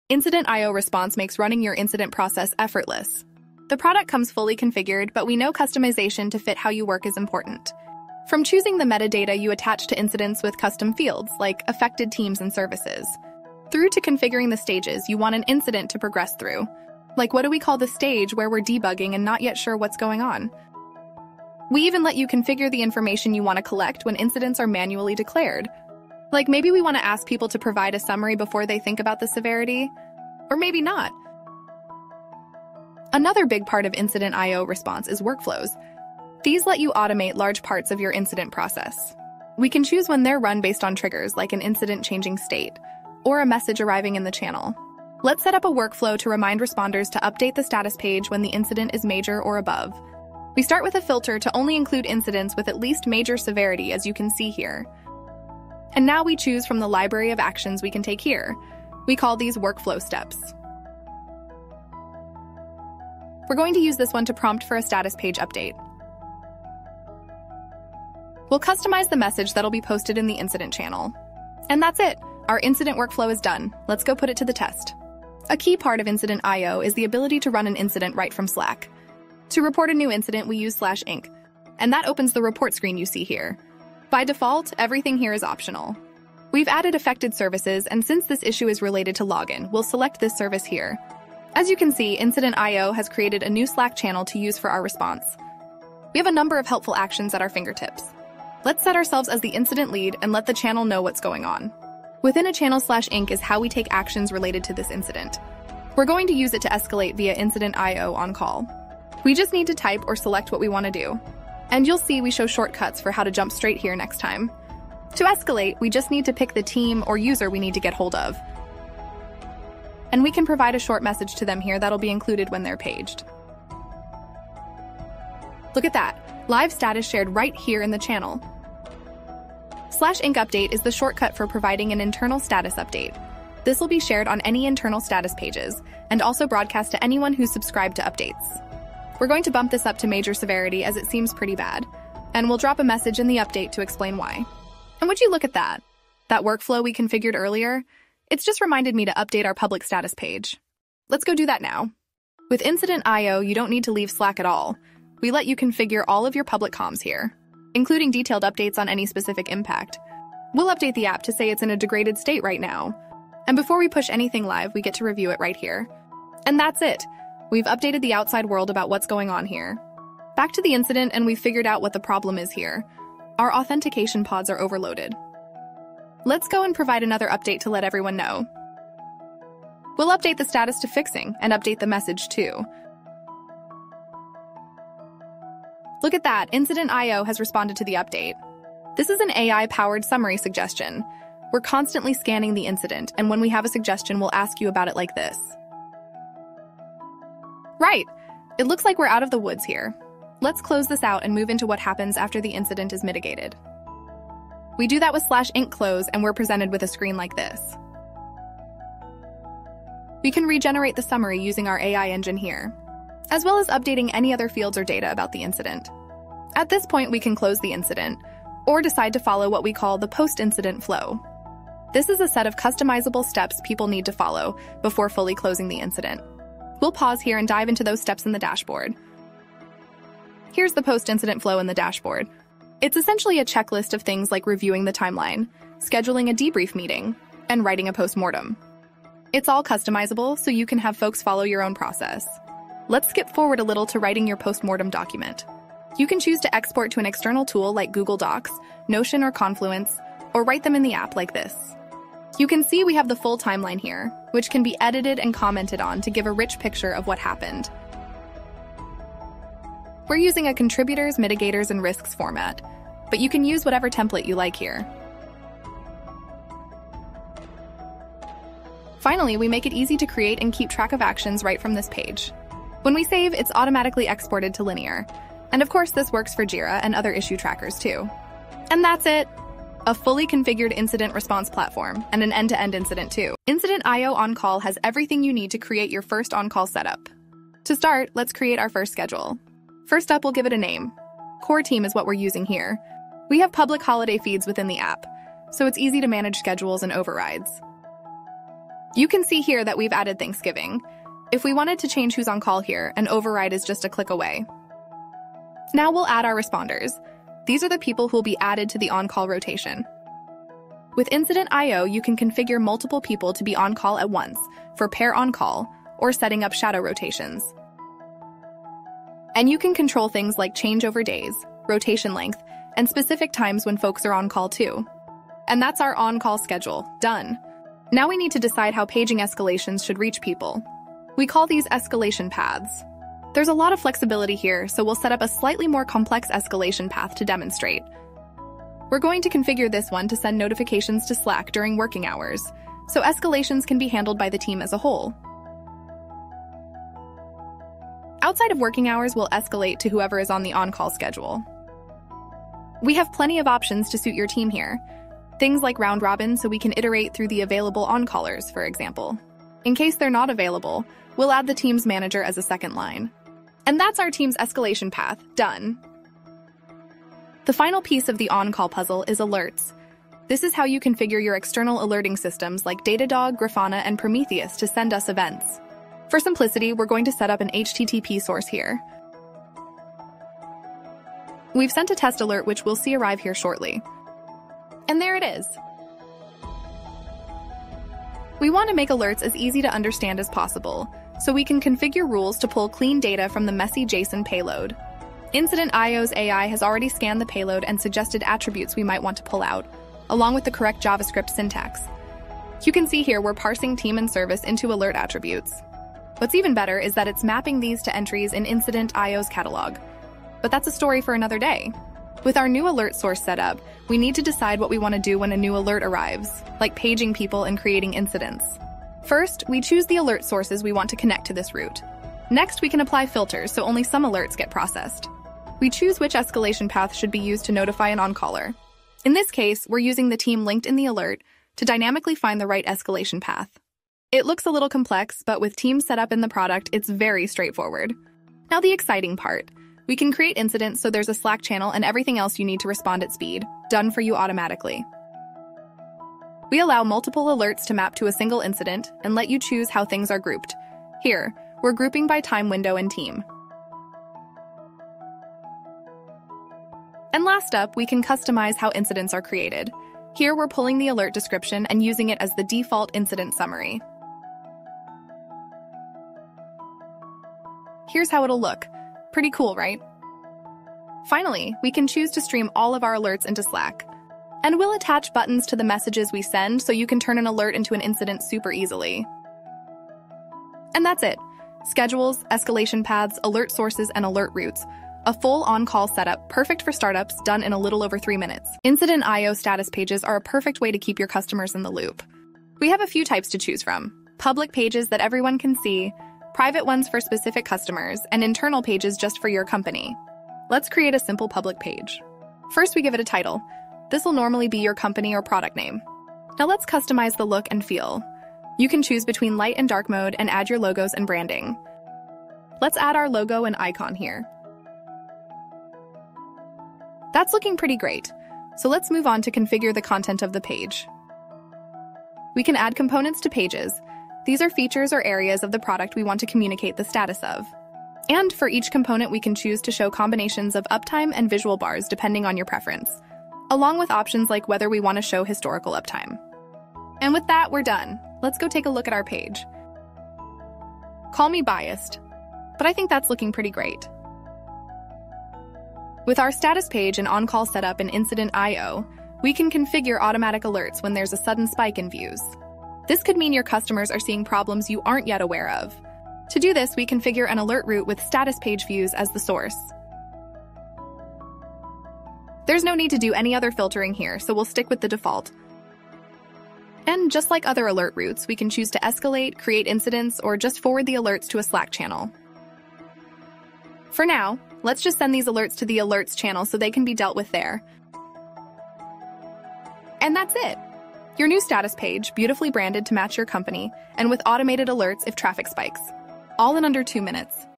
incident.io full platform walkthrough
A full walkthrough of incident.io Response, On-call and Status Pages.